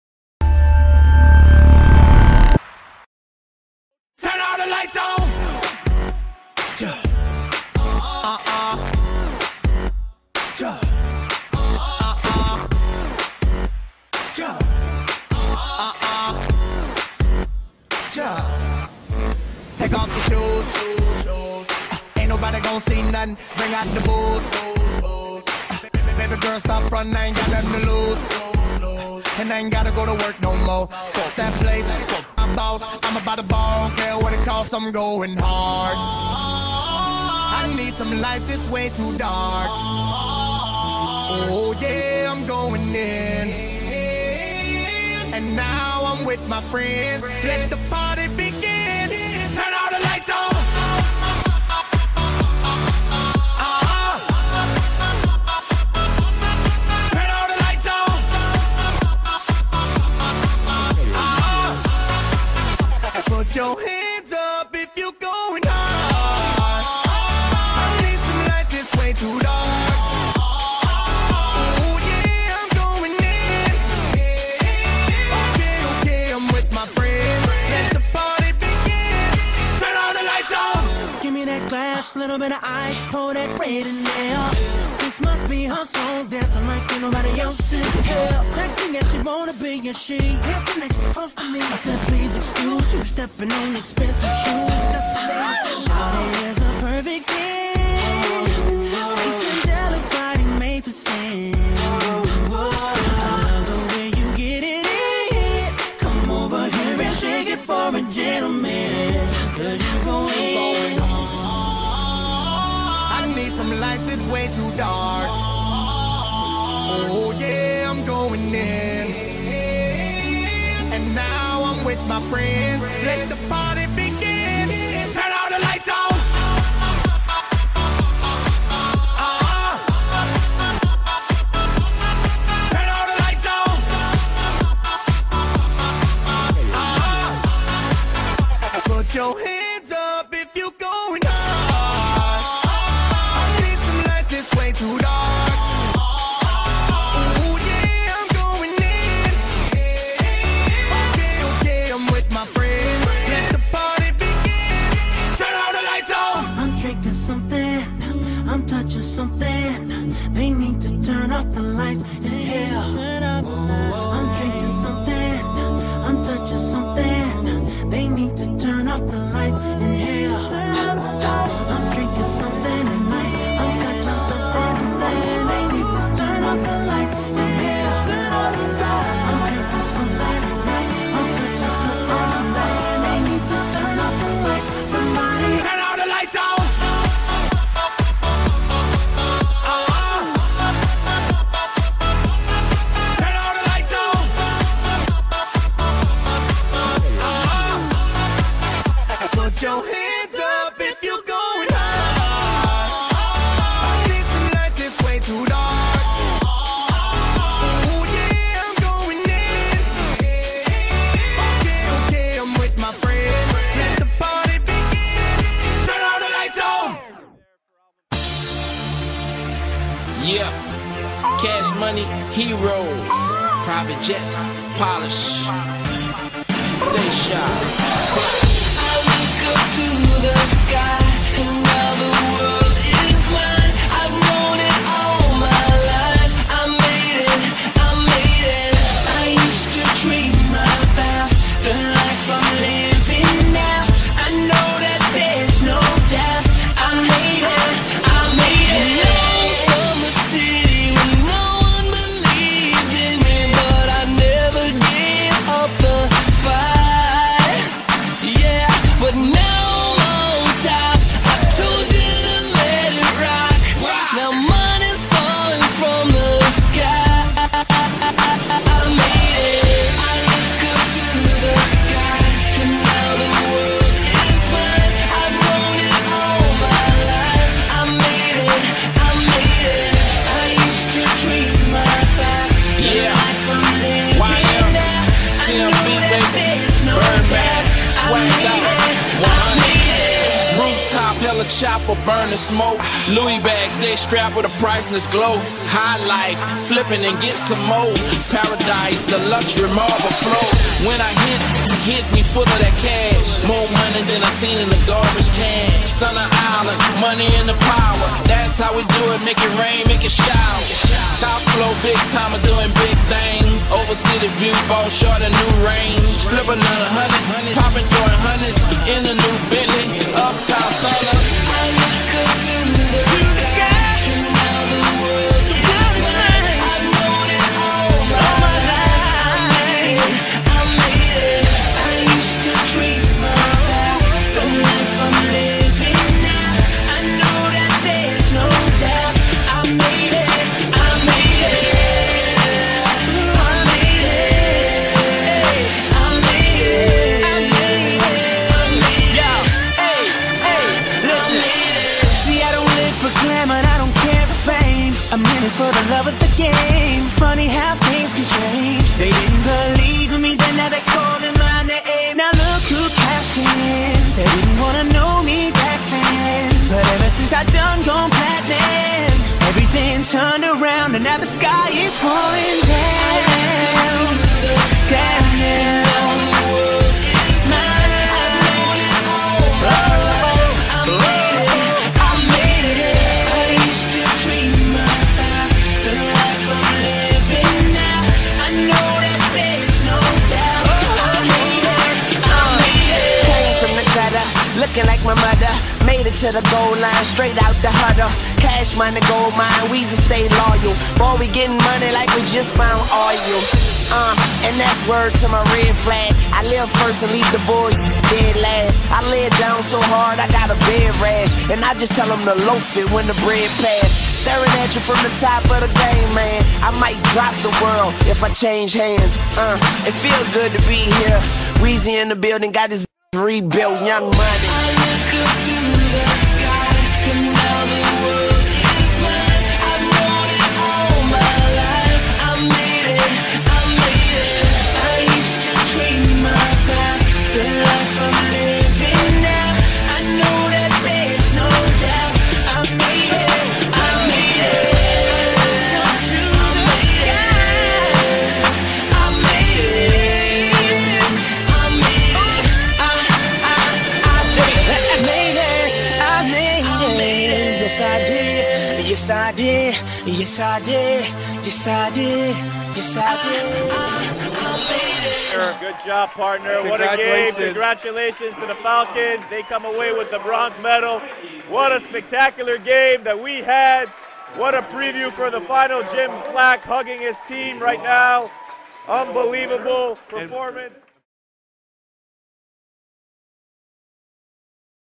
Langara Falcons 2013 CCAA Nationals Highlite Video
Highlites of the Falcons 4 games at the 2013 Nationals where they took home the bronze medal.